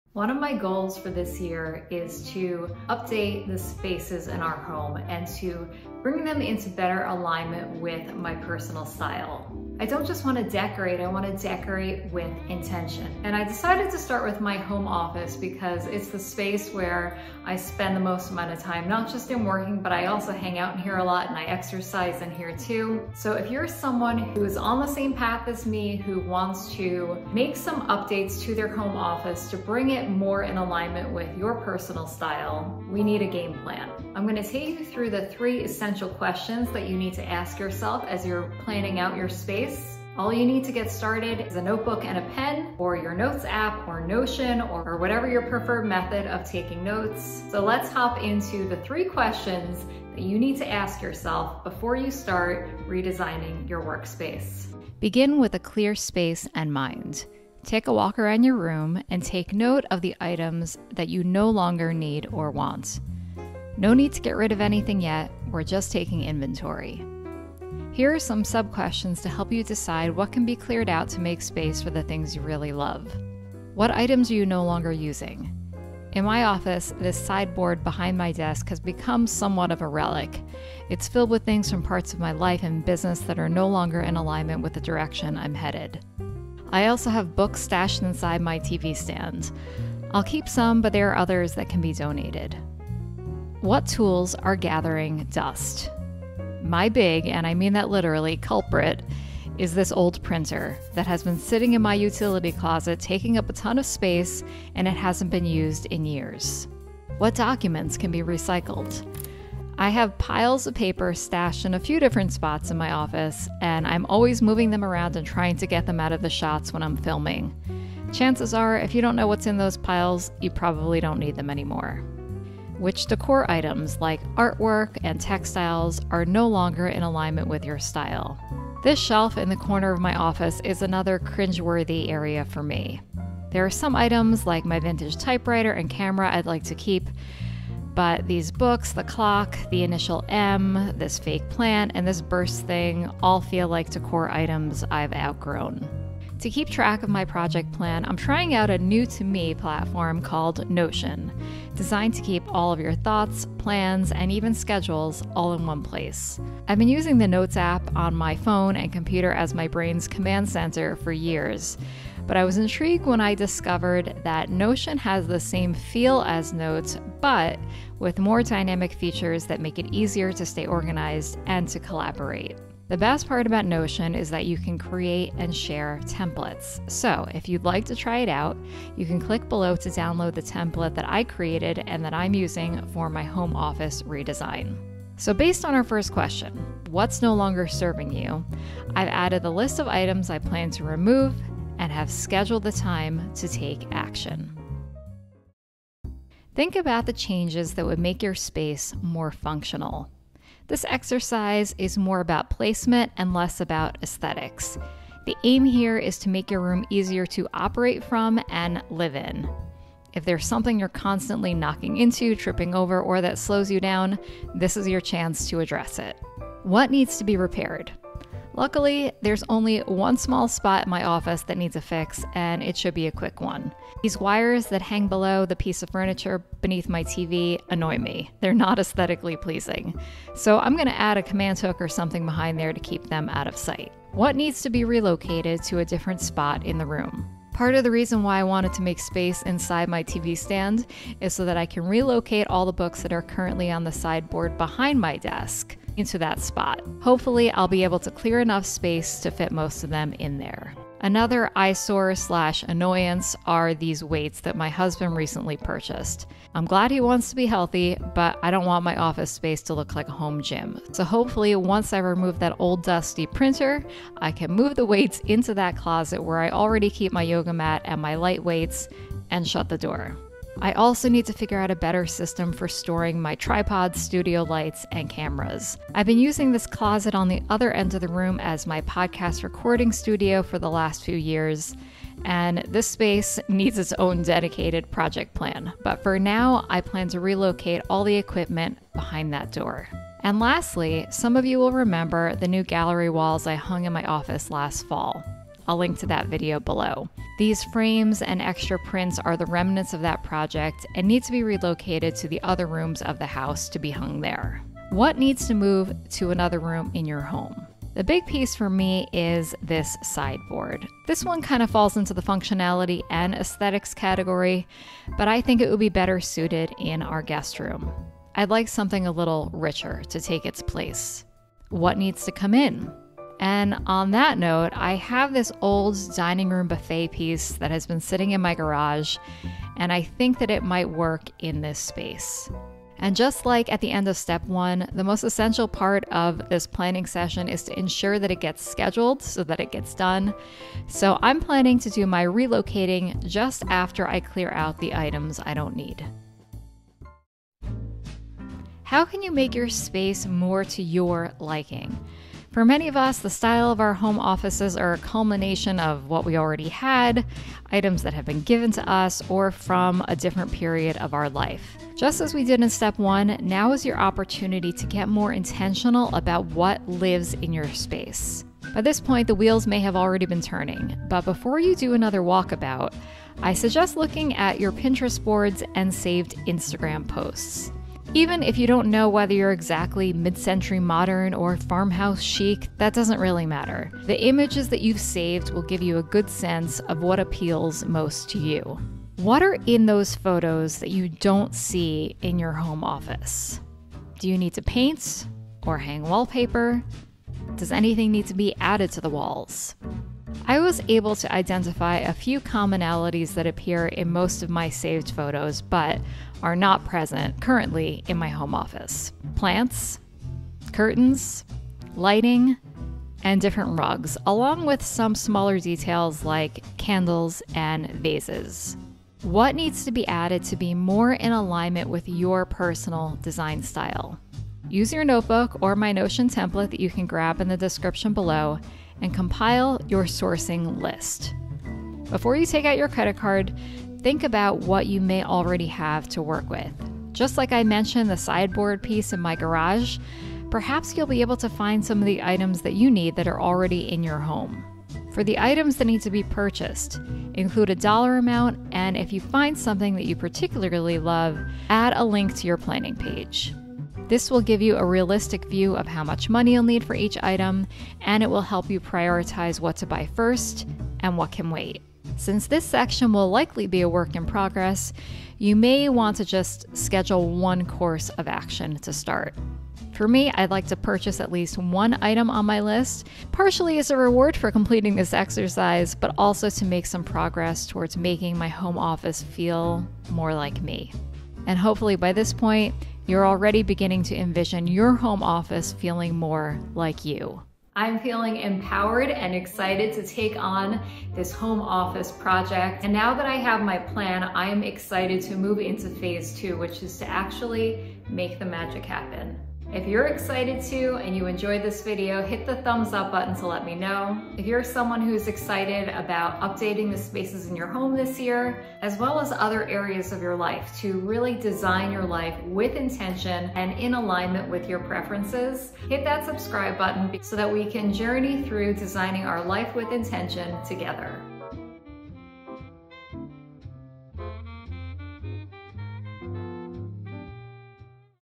Home Office Makeover Ideas 🎨🛠️✨How to plan it out
Are you looking for home office makeover ideas but feeling completely stressed by the process? Me too! In this video, I'm walking you through the three-step process I'm using to plan my office redesign.